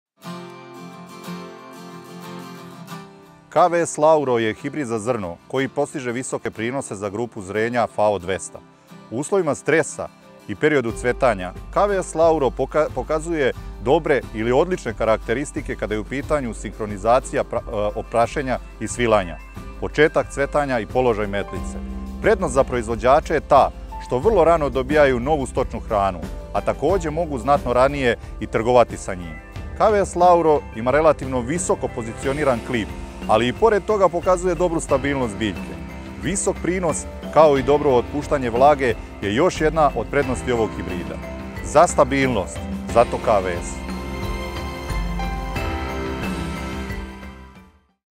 Rani hibrid za visoke prinose - Novi KWS hibrid kukuruza - KWS LAURO
NOVI ClimaCONTROL3 hibrid kukuruza – 🌽KWS LAURO🌽 – Rani hibrid za visoke prinose

KWS LAURO pripada FAO grupi 290 i odlikuje ga visok potencijal prinosa za grupu ranih hibrida. Ima veoma dobru stabilnost prinosa, u uslovima stresa ima sinhronizovano prašenje i svilanje.

Pratite nas na društvenim mrežama